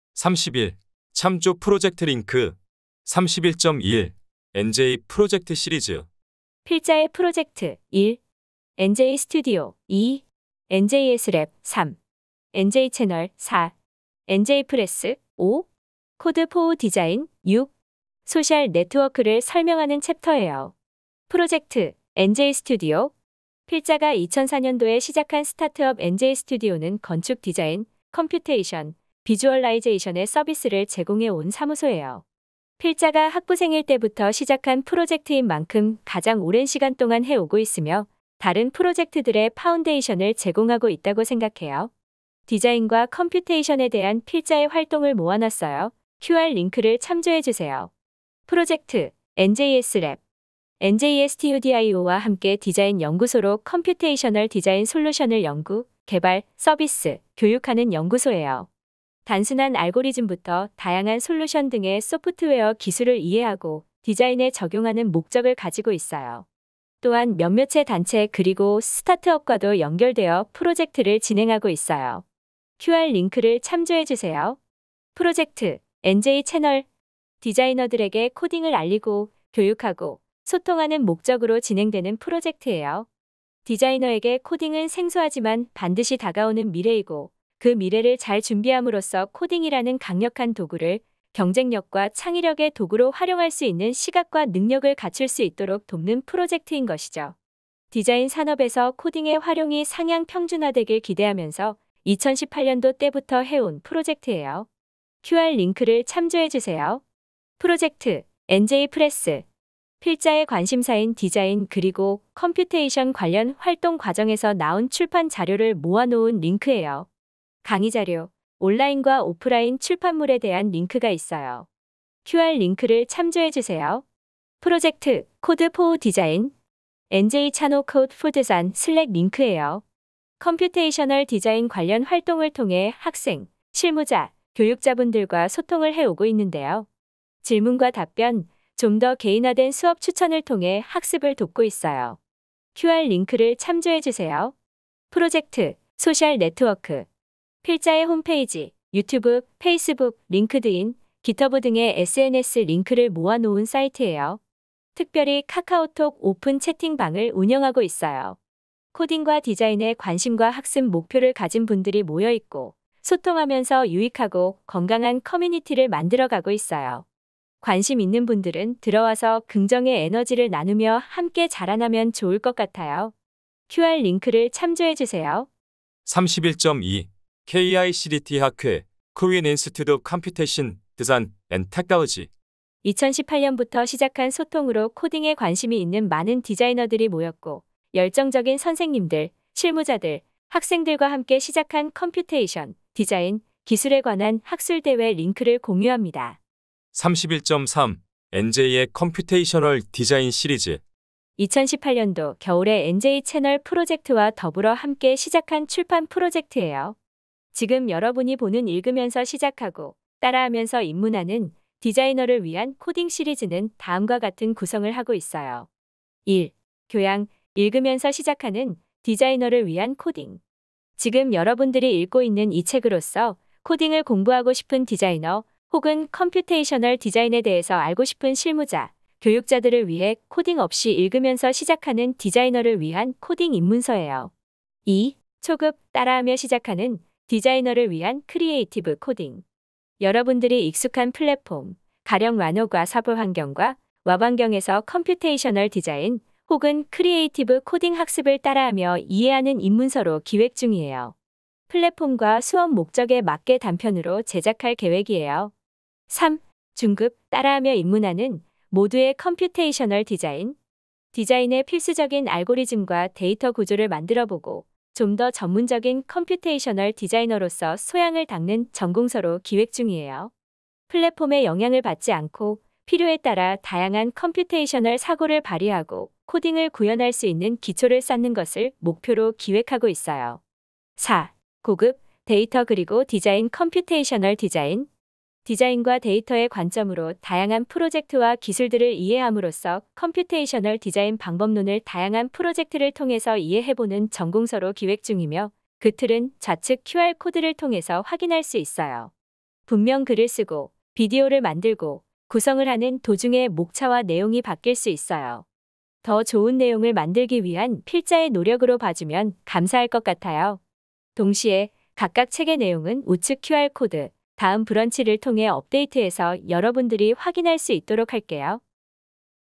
[컴퓨테이셔널디자인] 31/31 - 챕터8: 참조 프로젝트 링크
컴퓨테이셔널 디자인 (읽으면서 시작하는  디자이너를 위한 코딩) , 이남주 지음

00:00 - 31 참조 프로젝트 링크
00:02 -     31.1 엔제이 프로젝트 시리즈
02:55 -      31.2 KICDT 학회 Korean Institute of Computation, Design and Technology
03:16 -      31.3 엔제이의 컴퓨테이셔널 디자인 시리즈